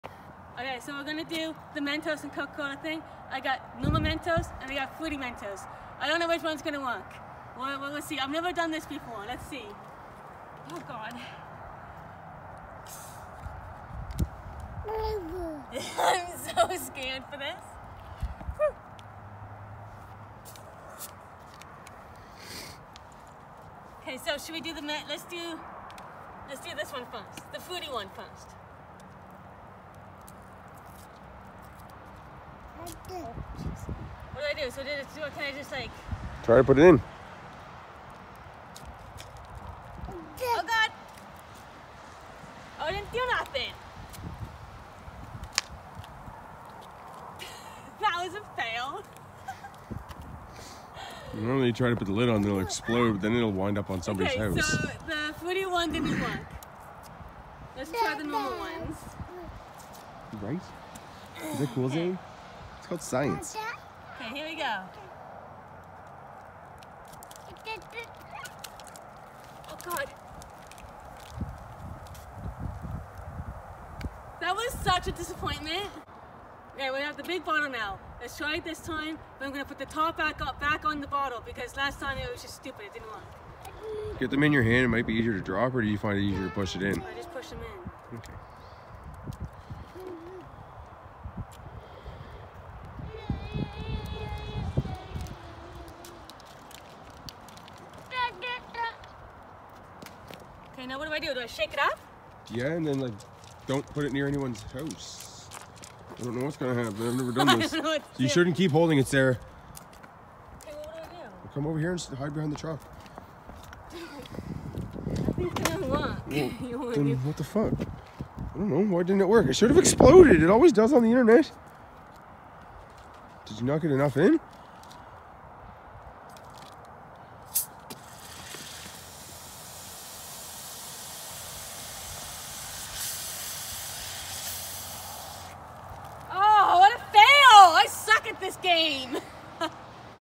First time EVER trying the coke and mentos thing. FAIL
This is my first time trying this. I failed so bad. I had no clue what I was doing.
I hope you enjoy this video of me failing at science 😂